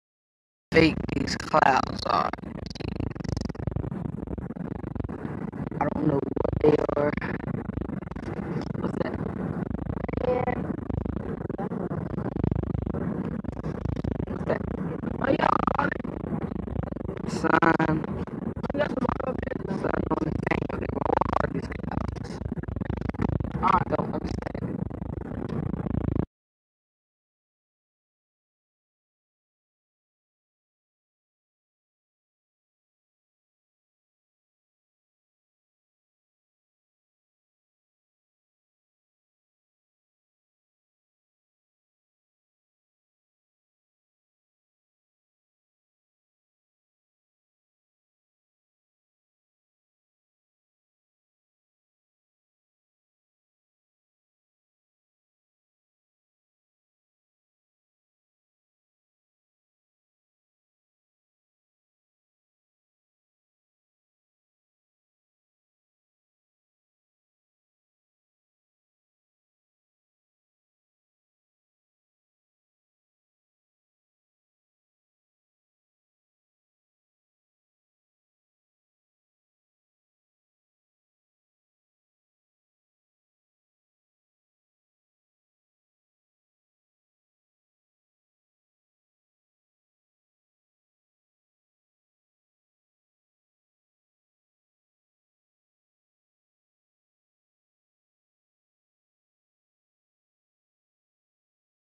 RAW: Footage of 2 Suns in Sky Caught on Film 11.01.17 !! Proof of 2 Suns
II was out recording and this is what happened..The flat earth is starting to look better everyday could be the largest star ever recorded